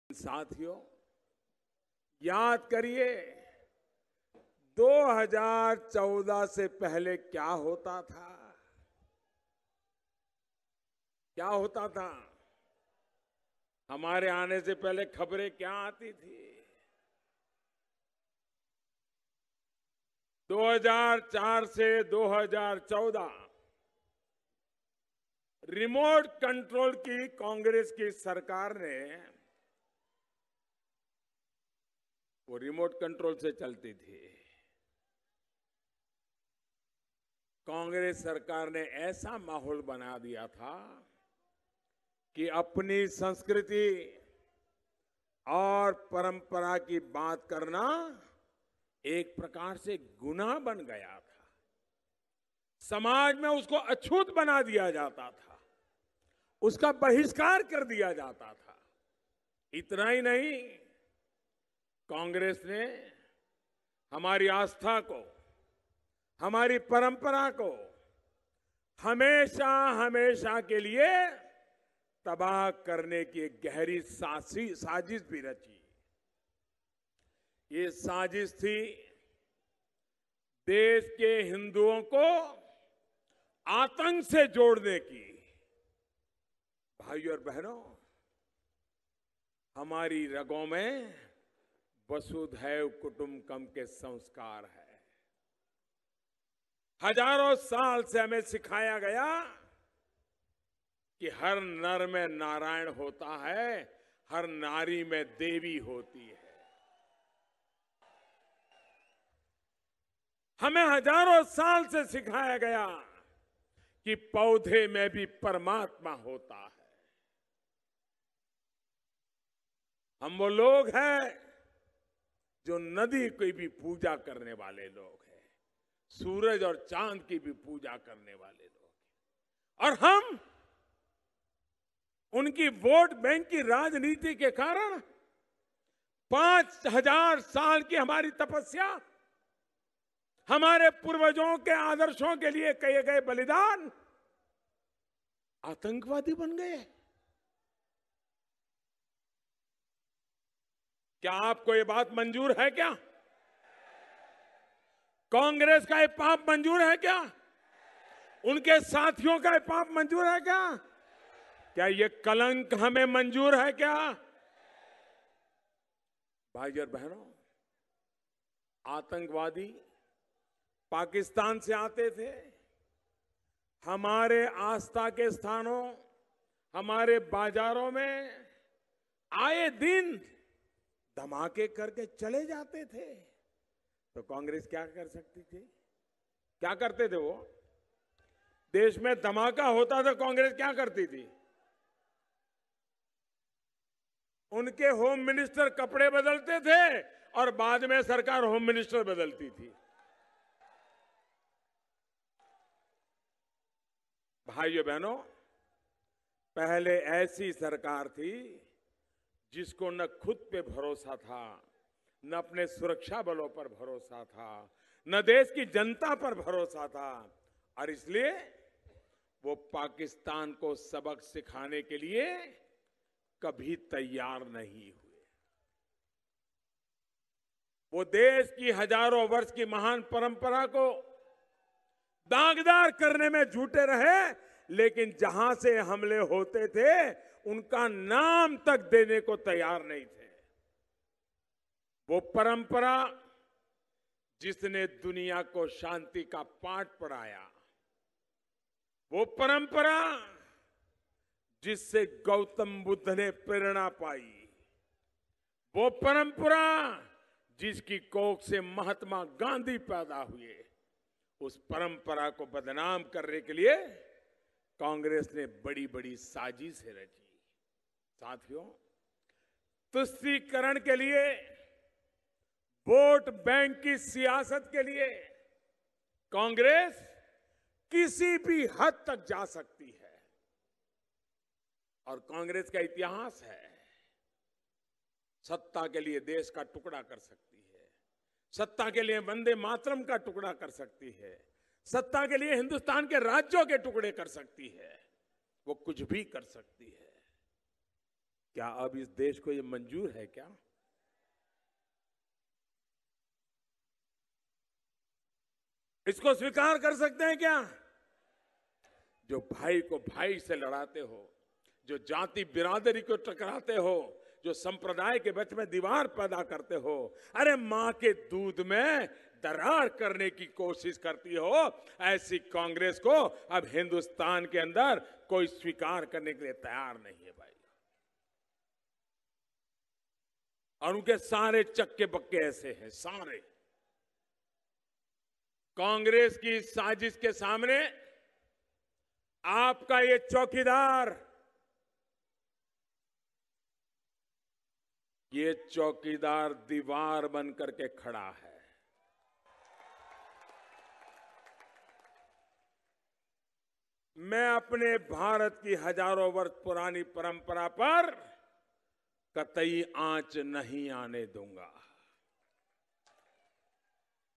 How Congress undermines India’s age old traditions for vote-bank politics? Find out in this video!
Prime Minister Narendra Modi addresses Public Meeting at Bhubaneswar, Odisha